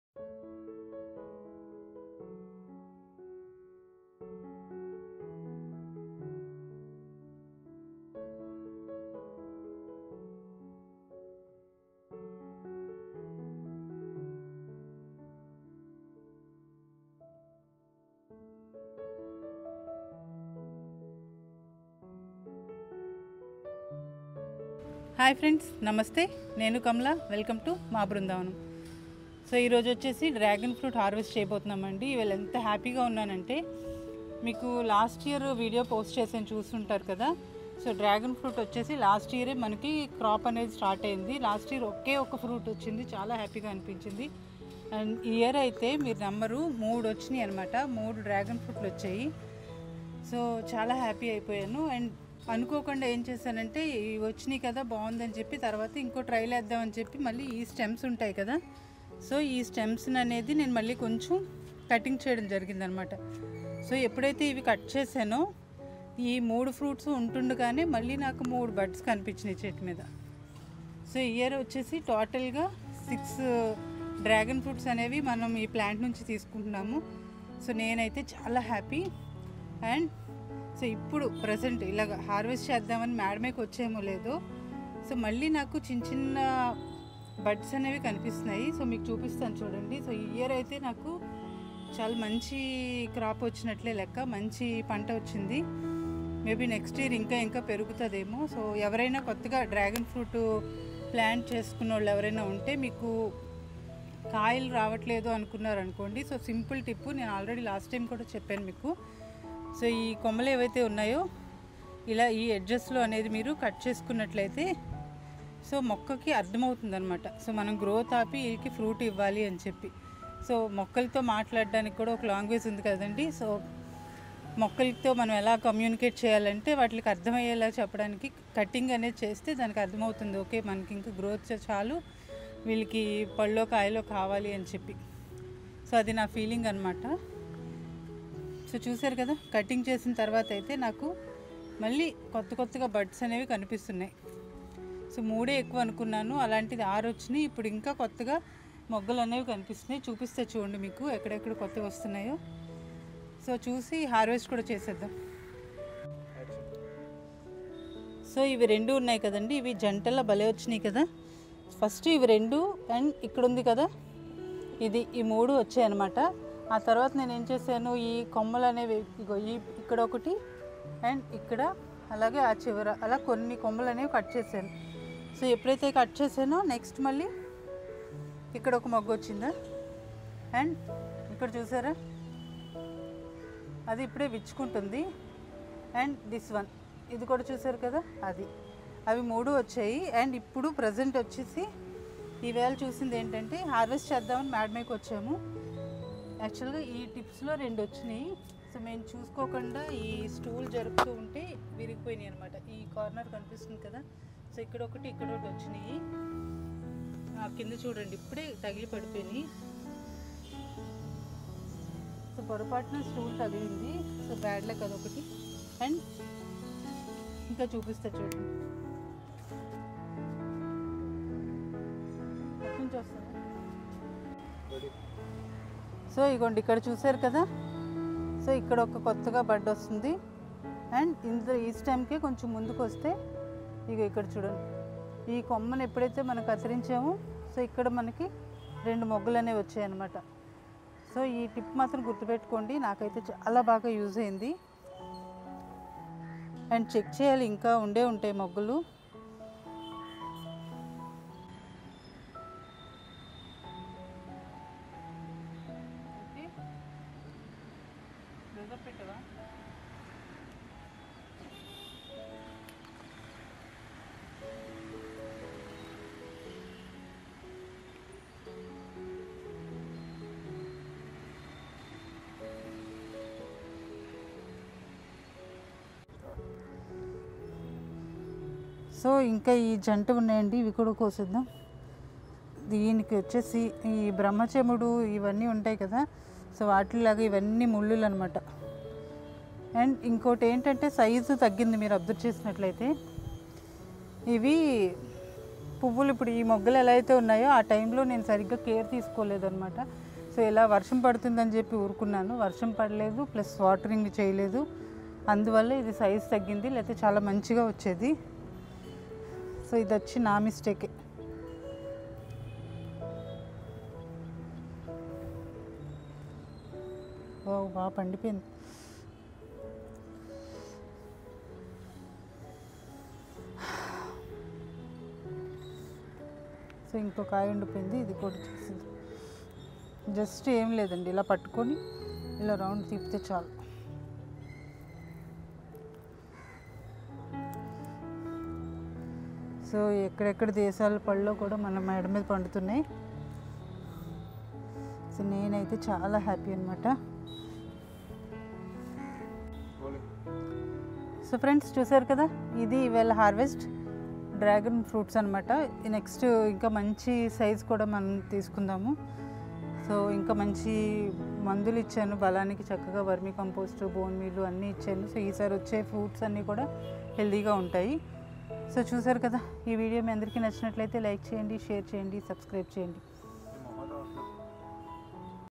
Dragon Fruit Harvest || Maa Brundavanam
Dragon fruit is high in vitamin C and other antioxidants, which are good for your immune system. It can boost your iron levels. Iron is important for moving oxygen through your body and giving you energy, and dragon fruit has iron. And the vitamin C in dragon fruit helps your body take in and use the iron.